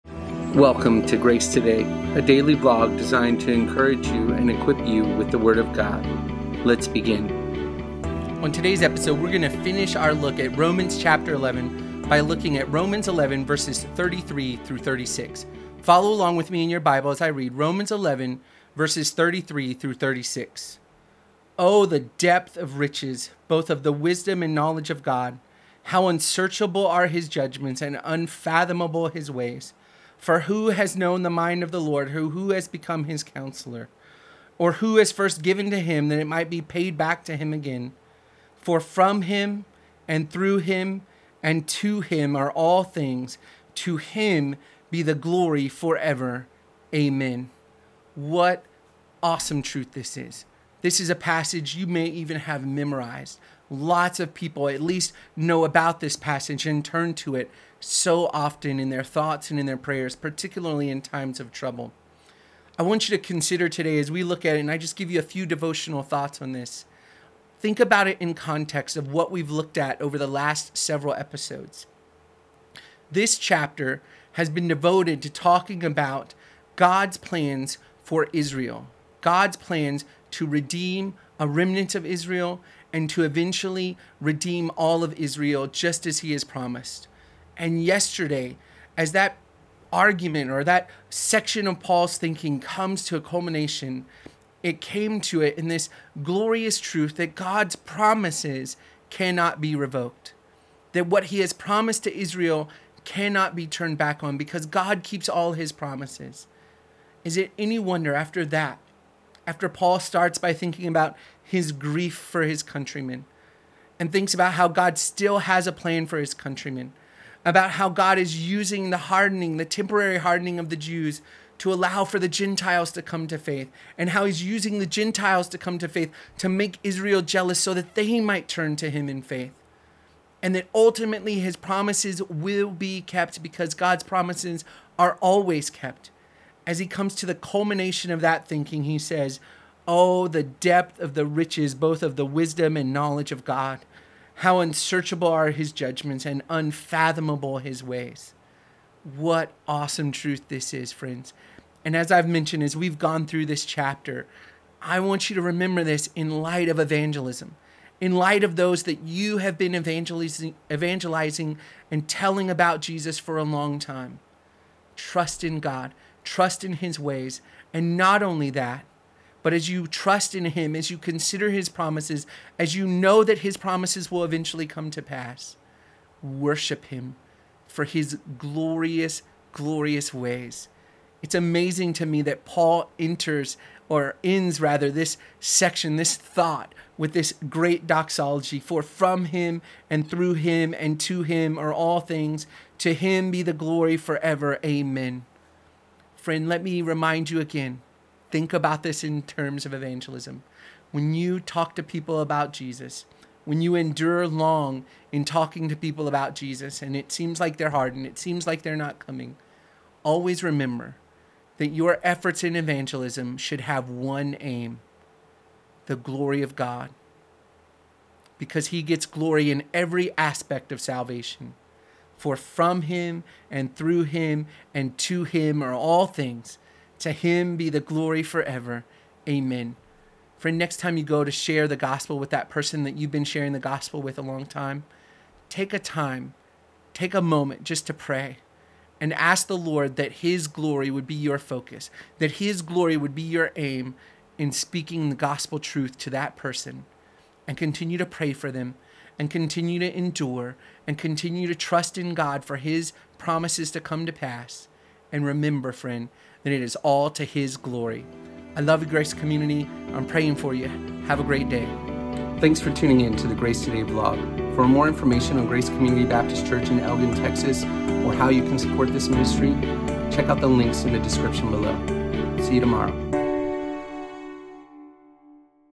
Grace Today: Rom. 11.33-36
Eric looks at Romans 11.33-36 and how God's plans for salvation, for Jews and Gentiles, brings Him glory.